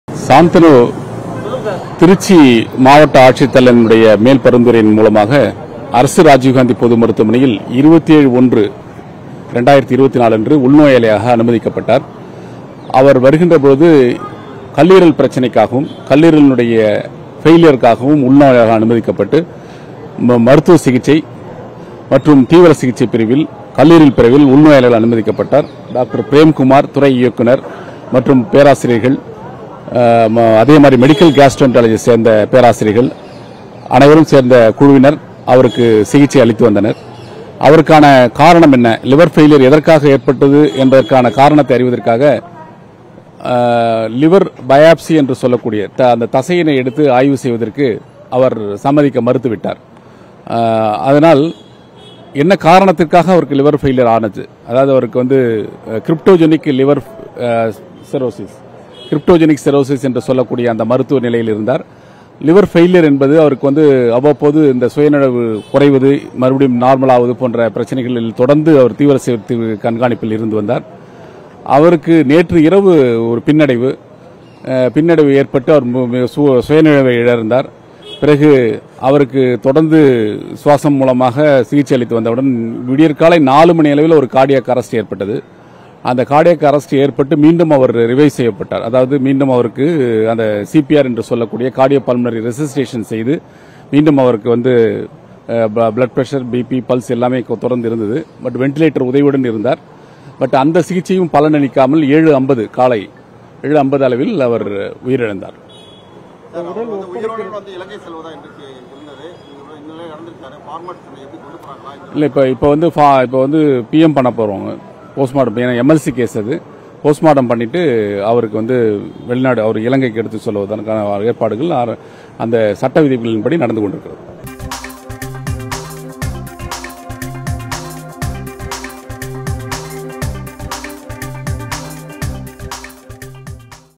சாந்தன் உயிரிழப்பு தொடர்பாக ராஜீவ் காந்தி அரசு பொது மருத்துவமனை டீன் விளக்கம்..!! Tamil News
"தொடர்ந்து அவரது உடல்நிலையில் பின்னடைவு இருந்தது"... ராஜீவ் கொலை வழக்கில் தண்டனை பெற்று உச்சநீதிமன்ற உத்தரவின் பேரில் விடுவிக்கப்பட்ட சாந்தன் சிறுநீரகம்- கல்லீரல் பாதிக்கப்பட்டு உயிரிழப்பு... ராஜீவ் காந்தி அரசு பொது மருத்துவமனை டீன் விளக்கம்..!!